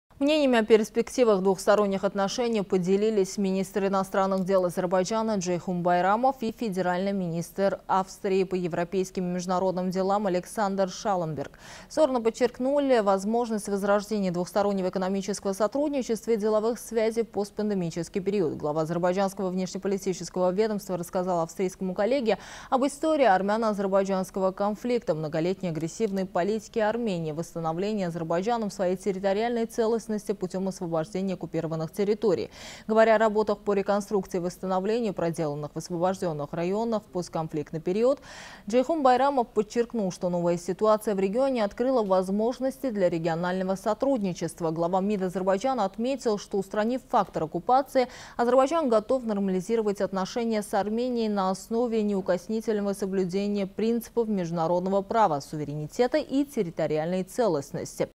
Глава МИД Азербайджана провел ряд встреч в рамках визита в Австрию
Мнениями о перспективах двусторонних отношений, поделились министр иностранных дел Азербайджана Джейхун Байрамов и федеральный министр Австрии по европейским и международным делам Александр Шалленберг. Стороны подчеркнули важность возрождения двустороннего экономического сотрудничества и деловых связей в постпандемический период. Глава азербайджанского внешнеполитического ведомства рассказал австрийскому коллеге об истории армяно-азербайджанского конфликта, многолетней агрессивной политике Армении, восстановлении Азербайджаном своей территориальной целостности путем освобождения оккупированных территорий. Говоря о работах по реконструкции и восстановлению, проведенных в освобожденных районах в постконфликтный период, Джейхун Байрамов подчеркнул, что новая ситуация в регионе открыла возможности для регионального сотрудничества